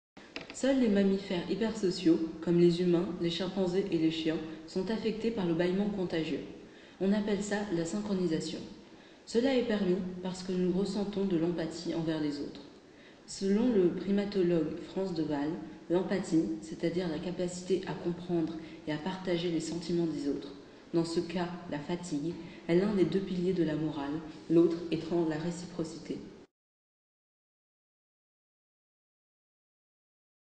L'empathie : du bâillement à la morale…
Courte animation réalisée dans le cadre du cours de philosophie par une élève de première. Automne 2015.